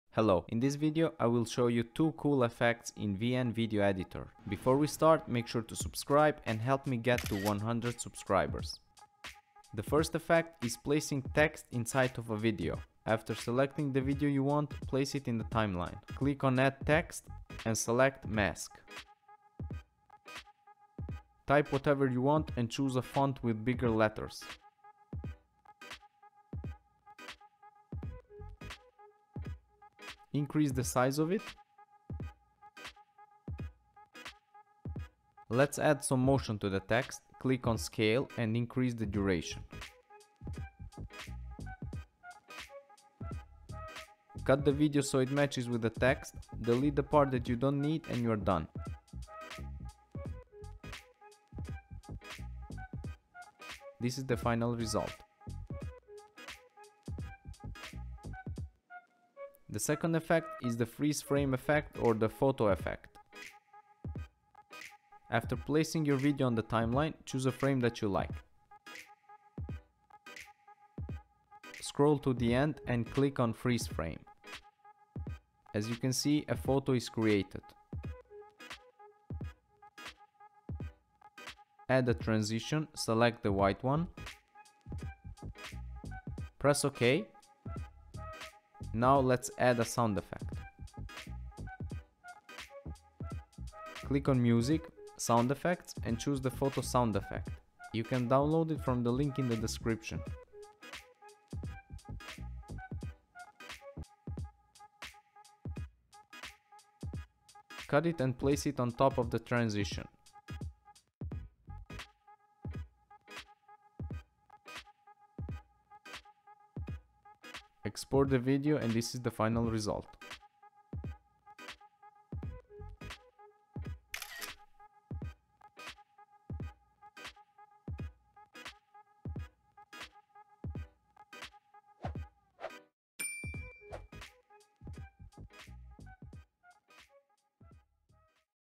2 Easy Effects in VN Video Editor Tutorial | How To | PC Windows 10/Mobile
In This Video I am Going To Show you 2 Cool and Easy Effects in VN Video Editor.
How to Put Video Inside of TEXT.
How to create a Snapshot Effect with Frame Freezing.